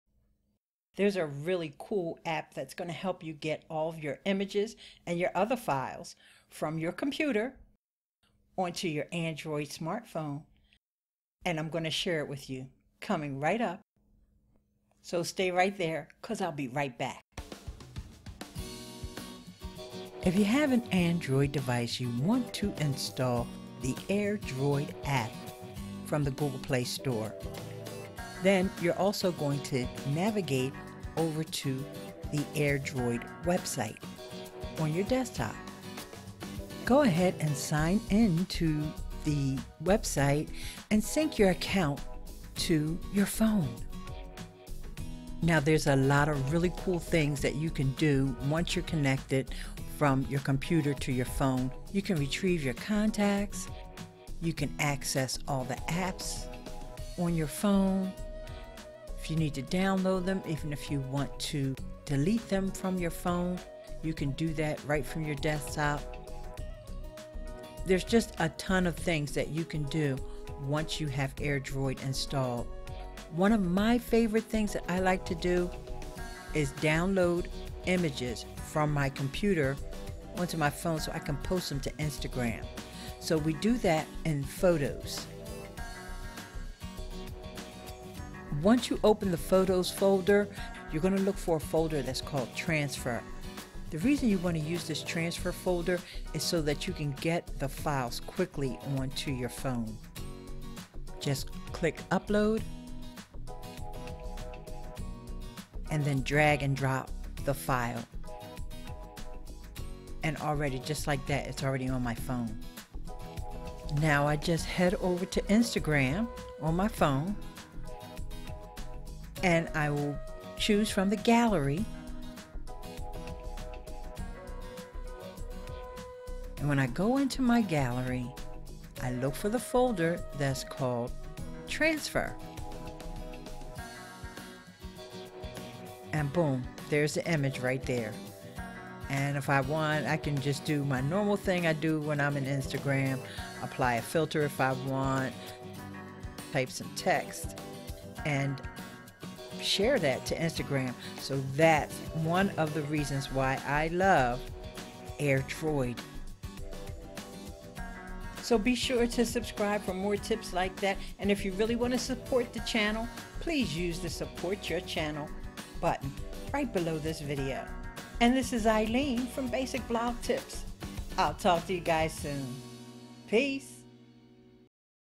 Post Photos to Instagram from Your PC with AirDroid Android App
The AirDroid App for Android connects your Android device to your phone remotely. You can use this remote connection to upload images from your computer to post on Instagram.

Transcript:

0:00 There's a really cool app that's going to help you get all of your
0:04 images and your other files, from your computer on to your
0:09 Android smart phone. And I'm going to share it with you coming
0:13 right up, so stay right there cause I'll be right back.
0:18 If you have an Android device you want to install the AirDroid app
0:26 from the Google Play store. Then you're also going to navigate
0:31 over to the AirDroid website on your desktop. Go ahead and sign in to the
0:39 website and sync your account to your phone. Now there's a lot of
0:46 really cool things that you can do once you're connected from your
0:50 computer to your phone. You can retrieve your contacts, you can access
0:55 all the apps on your phone, you can download them or even if you want
1:01 to delete them from your phone you can do that right from your desktop
1:07 There's just a ton of things that you can do once you have AirDroid installed.
1:13 One of my favorite things that I like to do is download images from my
1:18 computer onto my phone so I can post them to Instagram.
1:23 So we do that in Photos, once you open the Photos folder you going to look
1:31 for a folder that's called transfer. The reason you want to use this transfer
1:35 folder is so that you can get the files quickly onto your phone. Click Upload
1:42 and then drag and drop the file, and already just like that it's already on my phone.
1:54 Now I just head over to Instagram on my phone and I will choose from the Gallery
2:01 and when I go into my Gallery I look for the folder that's called Transfer.
2:14 Boom! There's the image right there. If I want I can just do my normal thing
2:21 I do when I'm in Instagram. Apply a filter if I want, type some text and share that
2:30 to Instagram. That's one of the reasons that I love AirDroid.
2:38 Be sure to subscribe for more tips like that and if you really want to support
2:42 the channel, please use the Support your channel button, right below this
2:47 video. This is Ileane from Basic Blog Tips I'll talk to you guys soon.
2:52 Peace!